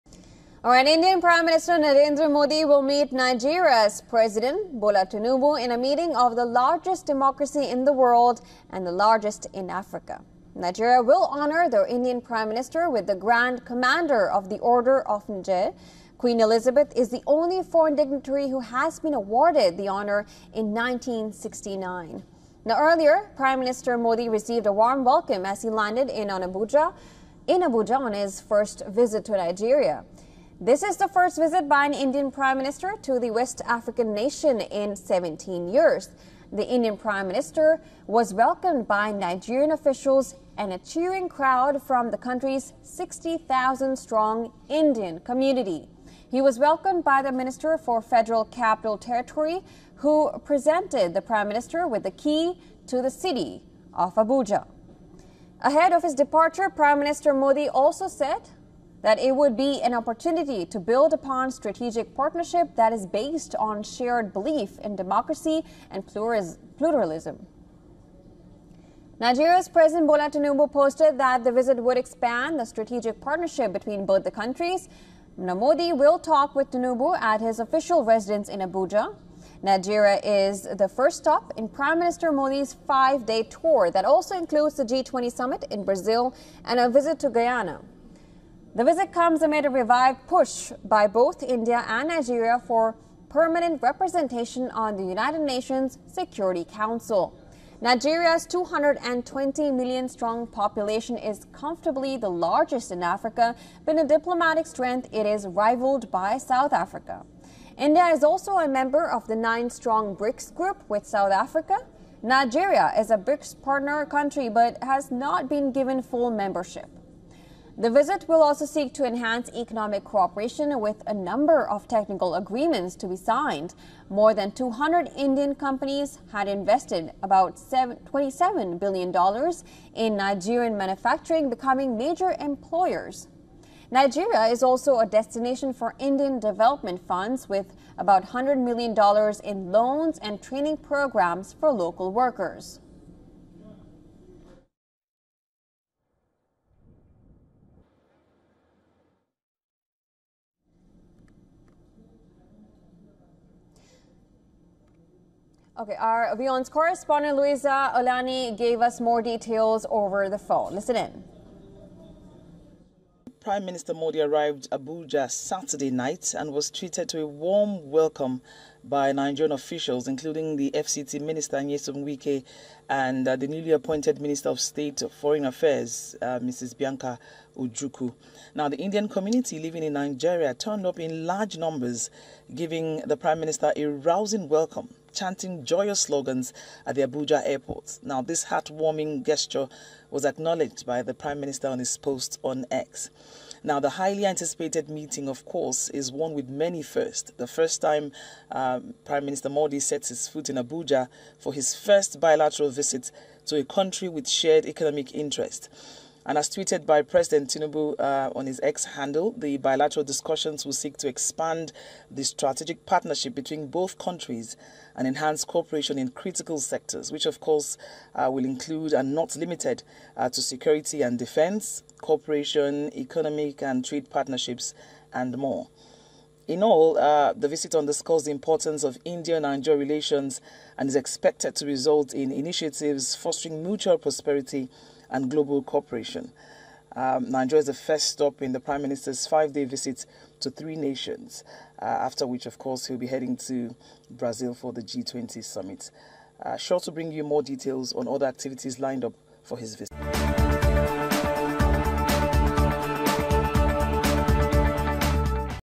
PM Modi’s Visit to Nigeria: Reinforcing Bilateral Relations After 17 Years | WION
Indian Prime Minister Narendra Modi's visit to Nigeria marks a significant step in strengthening bilateral ties between the two nations, after a 17-year gap. The visit aims to enhance cooperation in trade, security, and regional issues, while fostering deeper economic and diplomatic engagement.